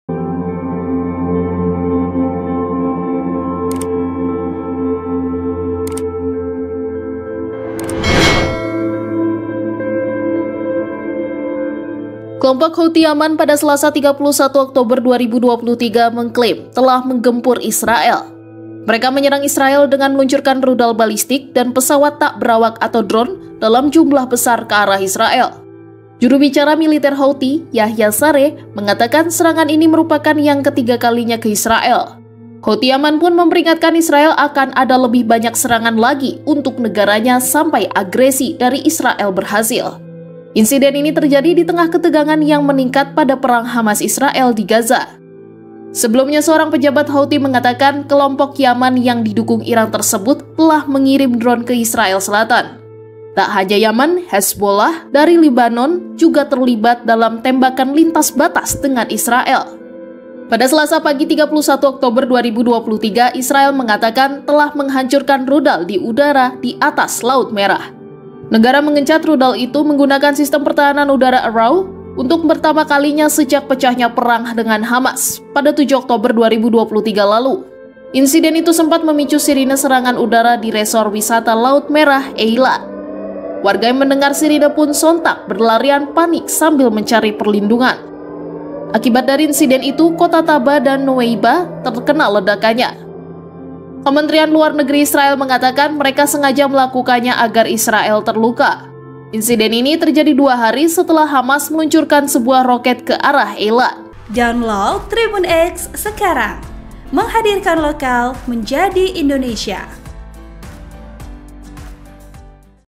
Houthi Yaman Luncurkan Rudal Balistik ke Israel
TRIBUNBANTEN.COM - Kelompok Houthi Yaman pada Selasa (30/10/2023) mengklaim telah meluncurkan rudal balistik dan pesawat tak berawak (drone) dalam jumlah besar ke arah Israel.

Mereka juga memperingatkan akan ada lebih banyak serangan lagi dengan menyasar wilayah Israel.

Juru bicara militer Houthi, Yahya Saree, mengatakan operasi ini adalah yang ketiga kalinya yang menargetkan Israel dan mengancam akan melakukan lebih banyak serangan lagi sampai agresi Israel berhenti.

Sumber: Kompas.com
Video Production : Rizki Asdiarman
Voice Over: Anneke Sherina Ramadhani